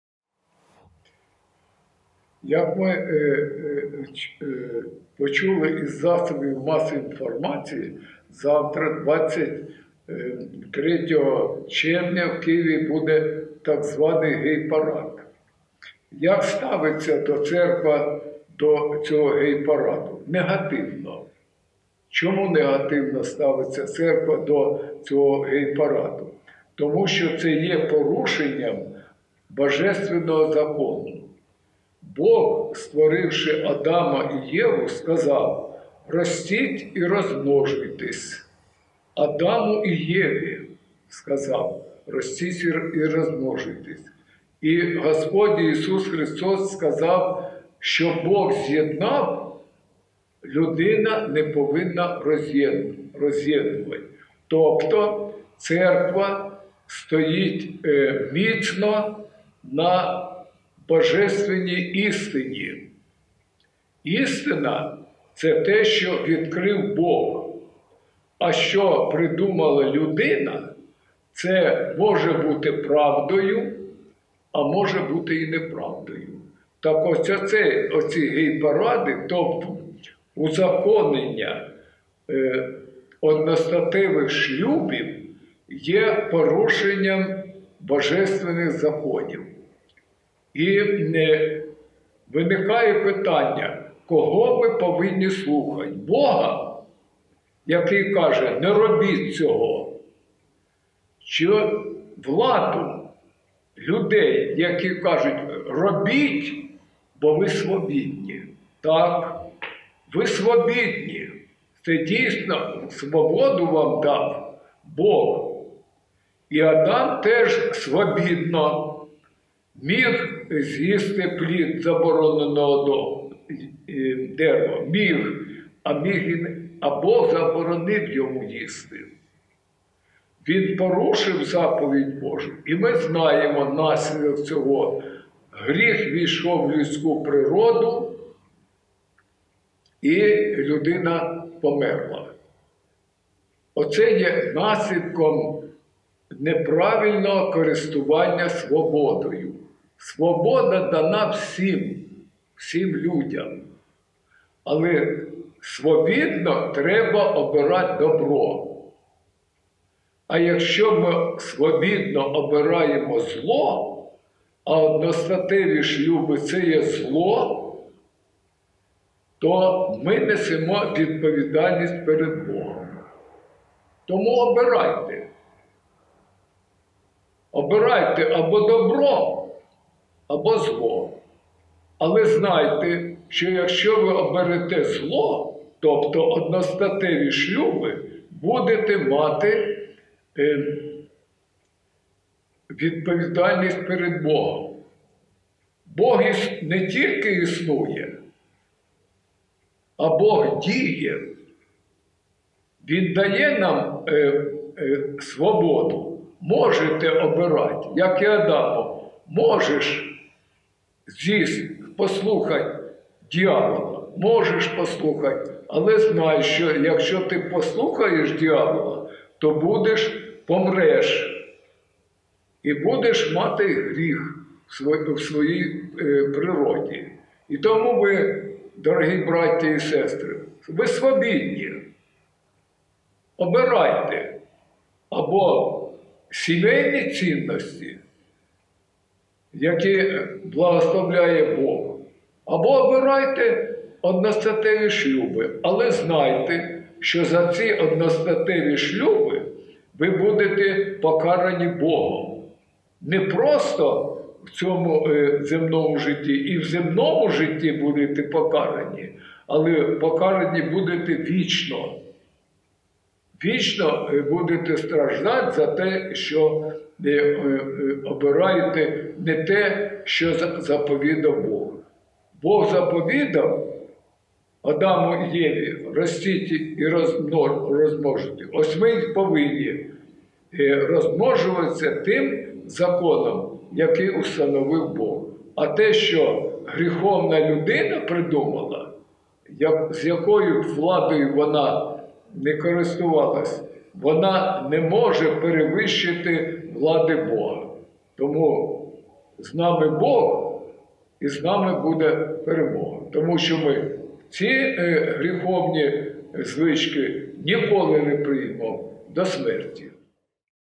Эксклюзив. Філарет засудив проведення гей-парадів в Україні
Предстоятель Української православної церкви Київського патріархату Філарет заявив, що гей-парад є порушенням божествинного закону. Порушенням сімейних цінностей, сімейних традицій, побудованих на заповідях Божих. Інститут сім'ї, як союз чоловіка і жінки, був встановлений і освячений Самим Богом.